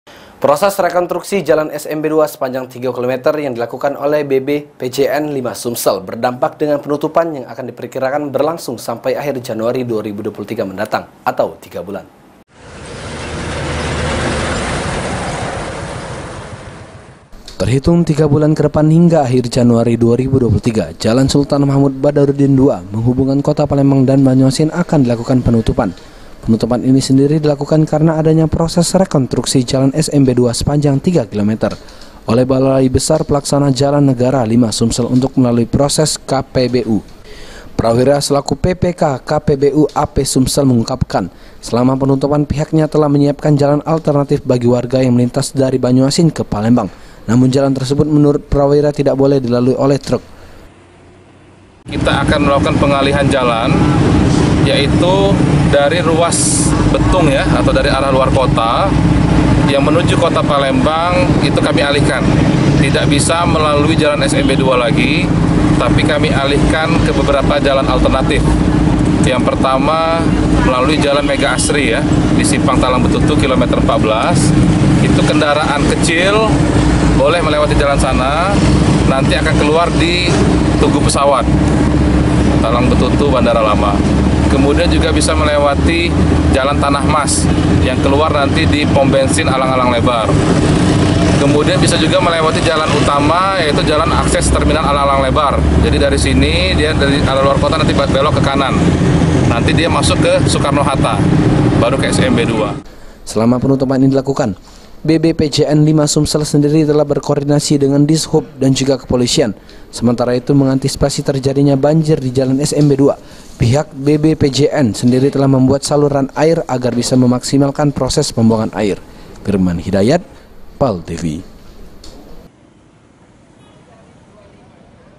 Penutupan Jalan Smb 2 Dilakukan Selama 3 Bulan | Paltv
Proses Rekontruksi  Jalan Smb 2 Sepanjang 3 Km // Yang Dilakukan Oleh Bbpjn 5 Sumsel / Berdampak Dengan Penutupan / Yang Akan Diperkirakan Berlangsung Sampai Akhir Januari 2023 Mendatang / Atau 3 Bulan